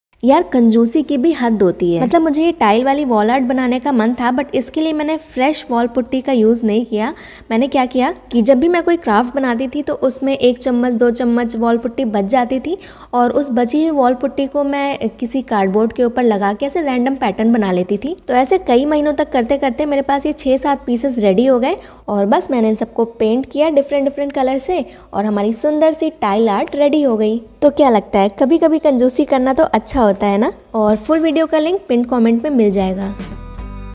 कभी-कभी कंजूस बनना पड़ता है
Hi friends.
In this video I am sharing how I Made 4 trendy AESTHETIC home decors in zero rupees using waste Materials.

Gear used-

•RING LIGHT
•BOYA MIC

MICRON PEN

CRAFT KNIFE CUTTER

Paper CUTTER

FEVICOL SH

GLUE GUN

ACRYLIC COLORS

PAINT BRUSH

ACRYLIC PEARL COLOR

DIY frames Tutorial-. .